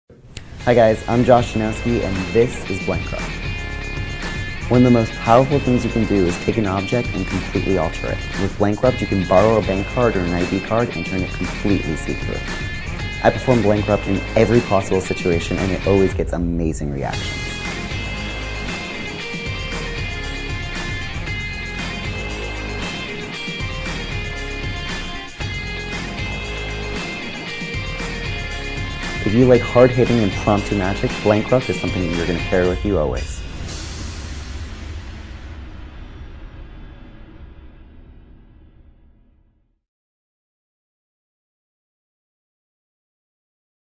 透明なクレジットカード / Blankrupt Thick Strip
お客様に借りたクレジットカードが透明になっちゃう！？マジックです☆

このマジックが気になった方は、マジック3を検索！